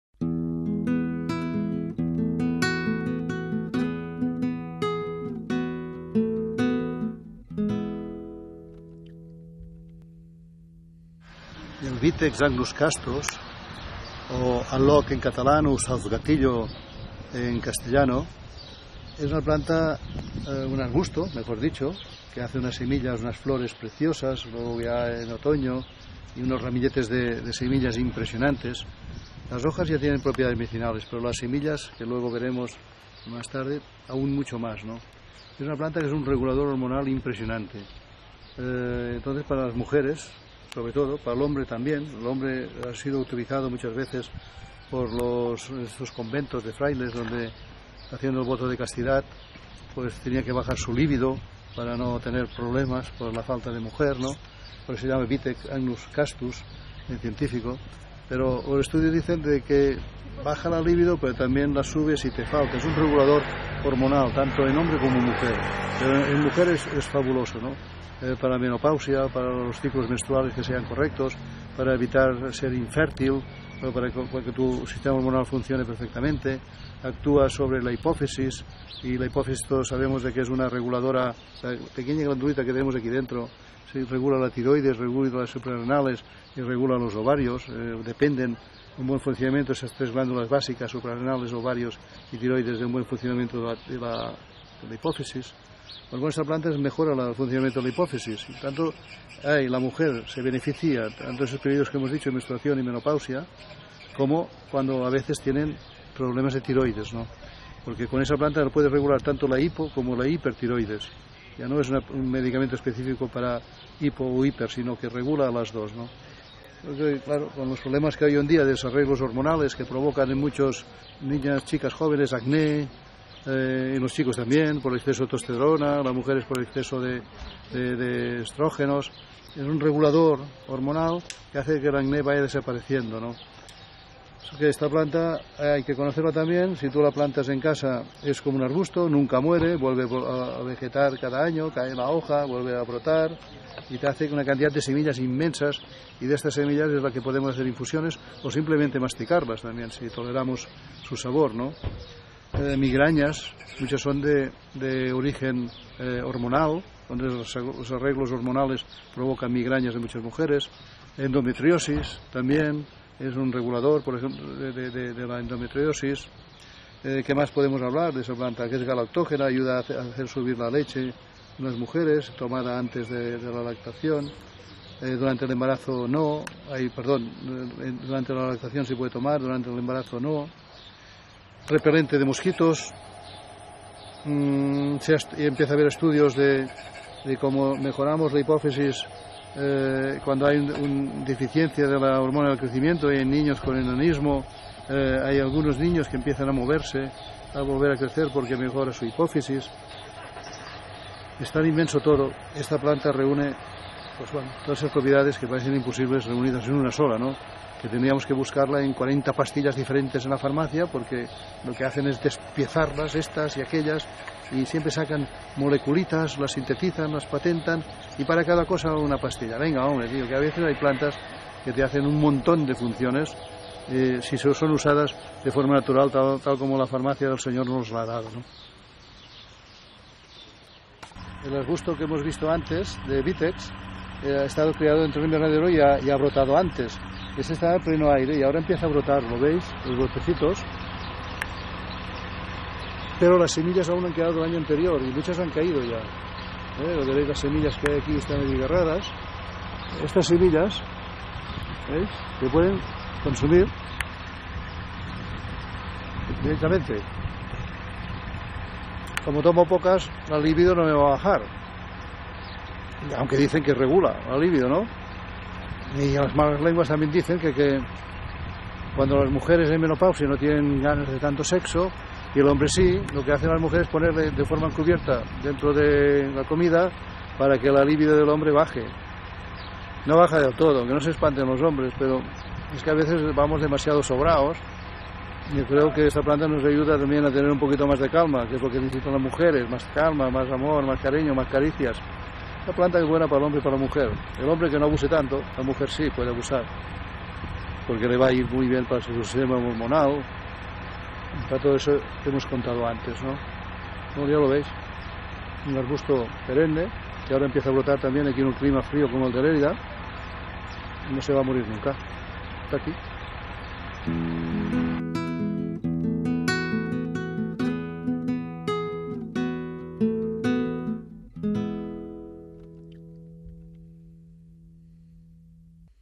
Vitex agnus castus Una planta Sagrada para la regulación hormonal
Vitex agnus-castus es una planta comúnmente conocido como sauzgatillo. Lo de “agnus-castus” (árbol casto) se debe a que originalmente fue usada como medicina “antilíbido” por monjes para ayudarlos en su celibato.

La planta de Vitex rebrota cada año y produce una infinidad de semillas. Podemos tomarlas mediante infusión o masticarlas directamente.

Estas bayas son consideradas tónicas para el sistema reproductor masculino y femenino. También son beneficiosas para los desarreglos hormonales y para el síndrome premenstrual y otros trastornos ginecológicos. Eso sí, se aconseja no tomarlas durante el embarazo.

Debido a que ayuda a mantener los niveles de estrógeno en el cuerpo se la considera un buen remedio para el hipertiroidismo. Siempre es aconsejable comprobar los niveles de estrógeno en su cuerpo para asegurar que la verdadera causa de hipotiroidismo es el estrógeno

Tomar siempre con precaución y bajo la propia responsabilidad.